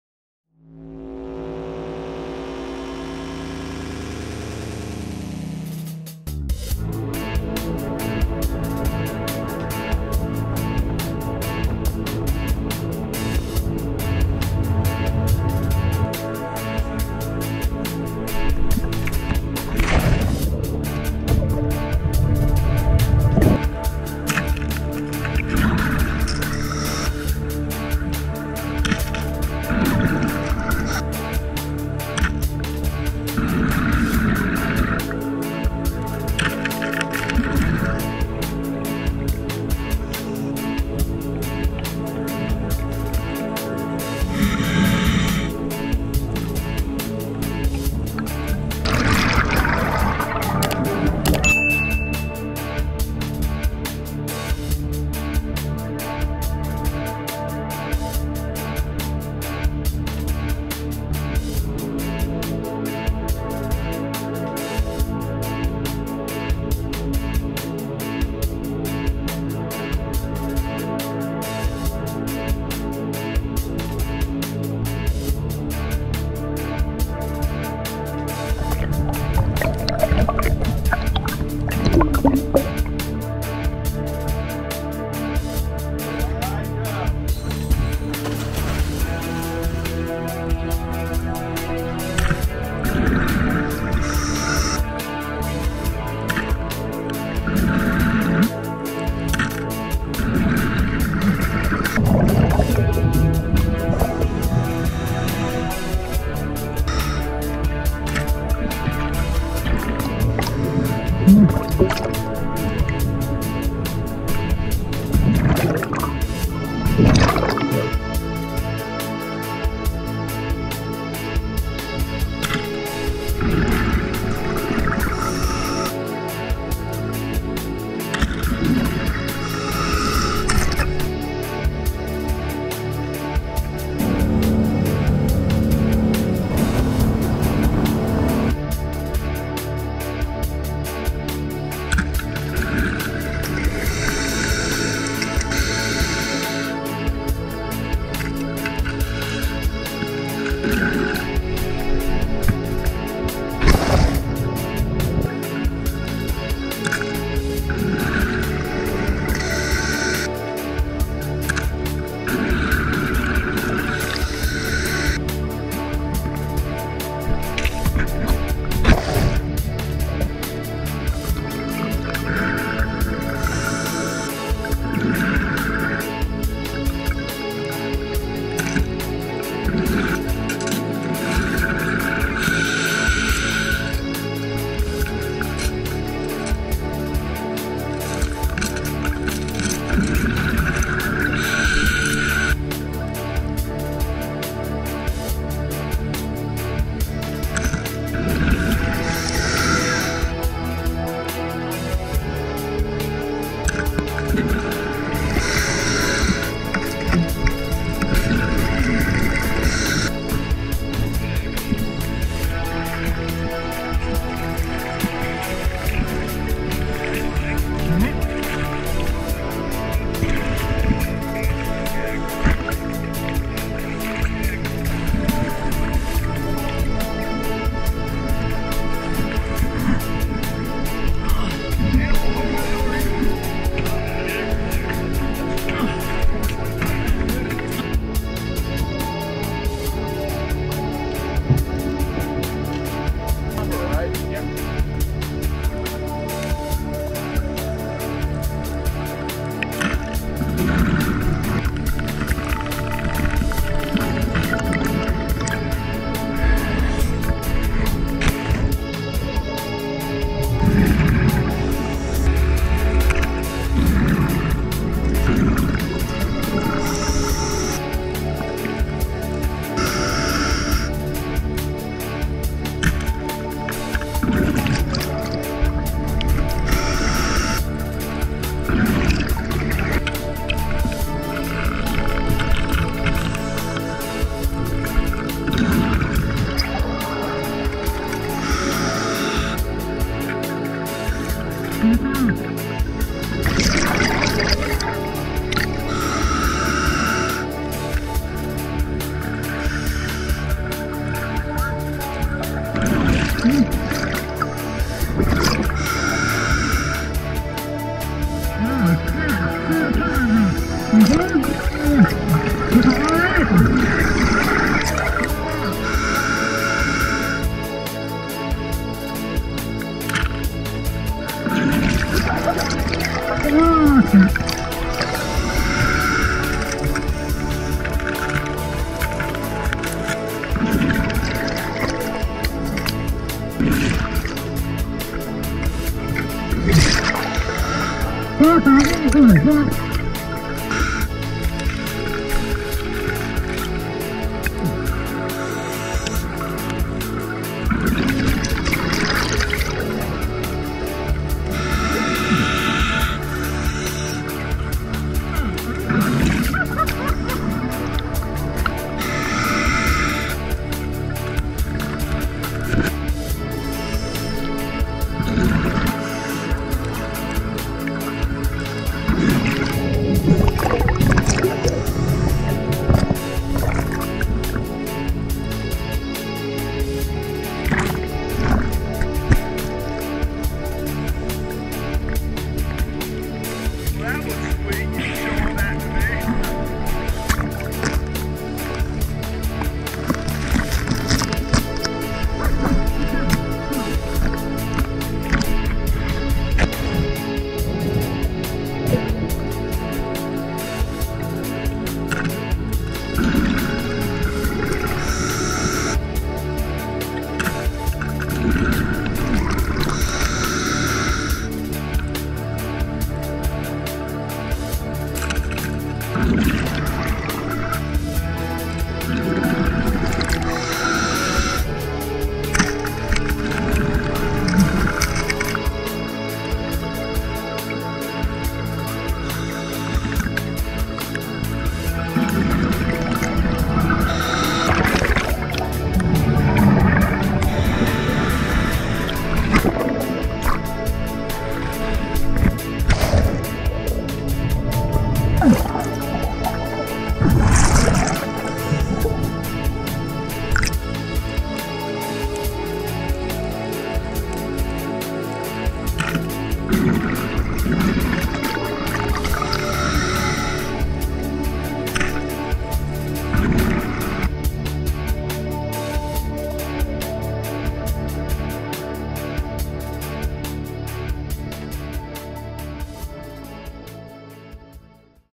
FLORIDA MIDDLE GROUNDS SPEARFISHING JUNE 2021
Footage from a 3-day commercial fishing trip with @danmacmahon3973 off the HEADHUNTER 44 to the Florida Middle Grounds in the Gulf of Mexico in June 2021.